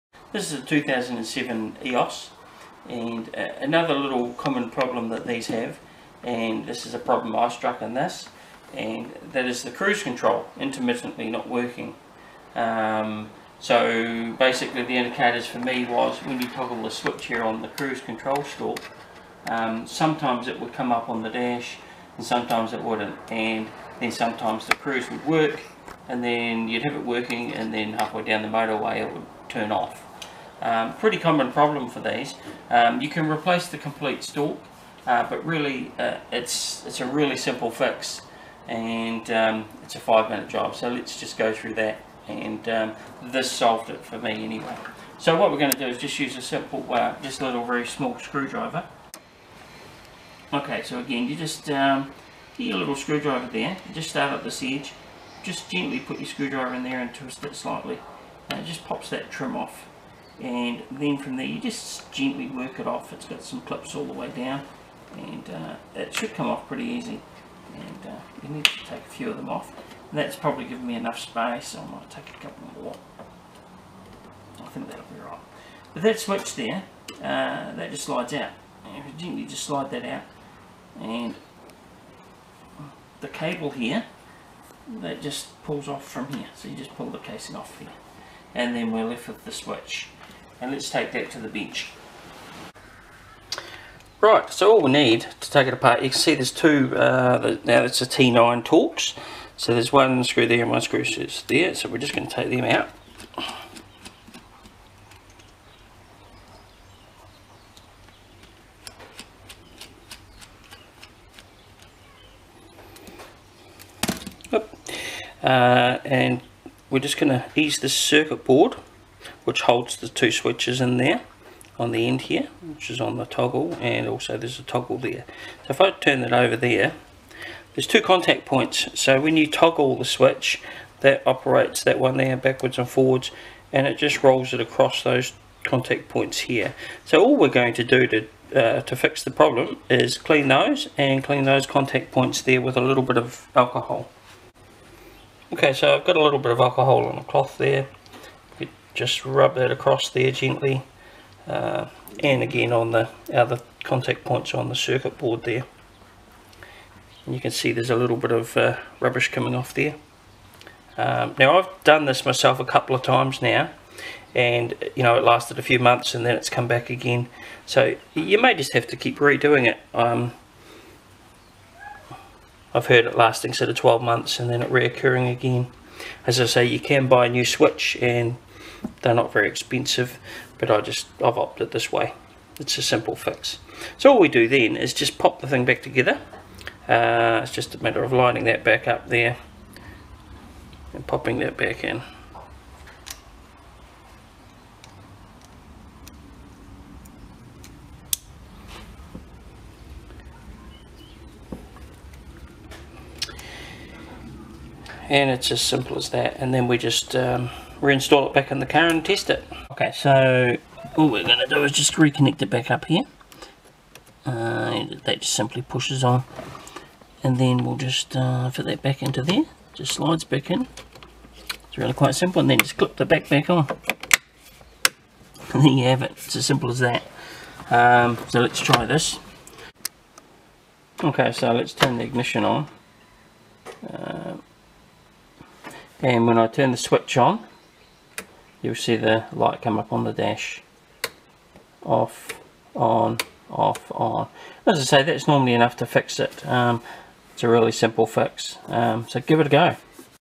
VW EOS fixing cruise control
5 min easy fix for your EOS cruise control intermittently not working.